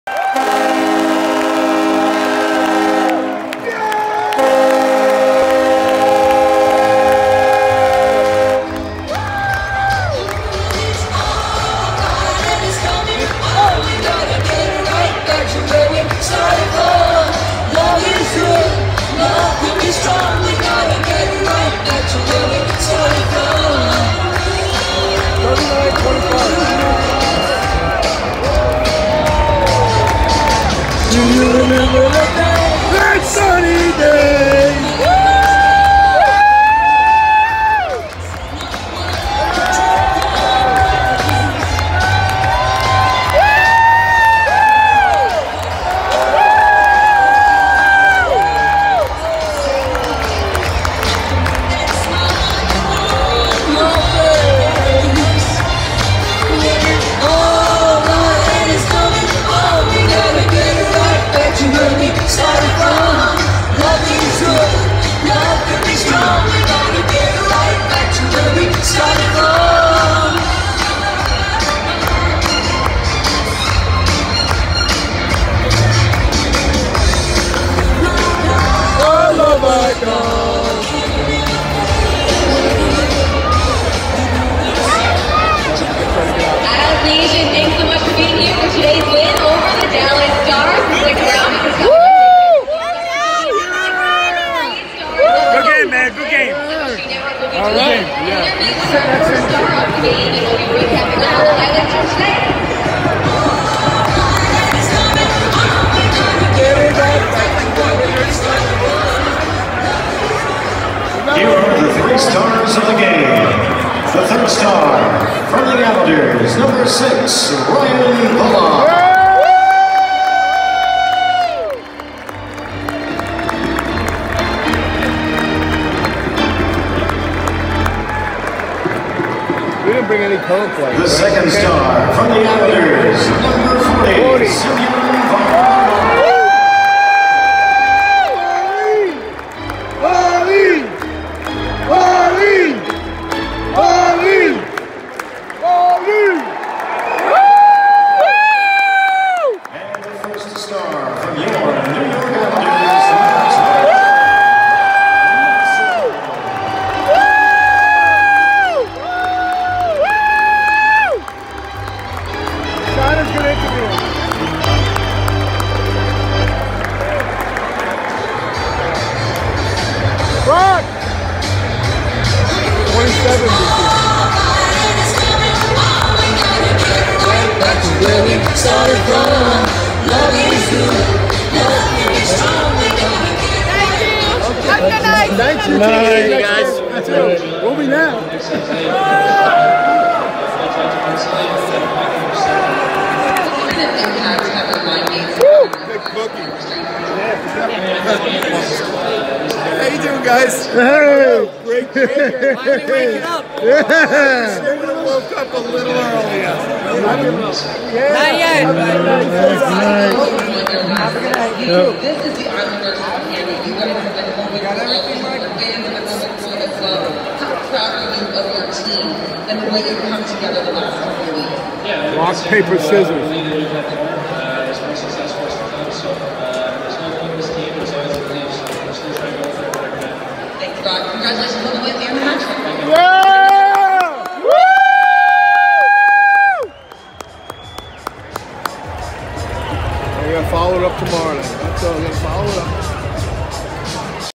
Post-Game Celebration & 3 Stars March 19 2022 Stars at Islanders UBS Arena
isles cap a 4-2 win over the Stars at UBS to go to 26-24-9.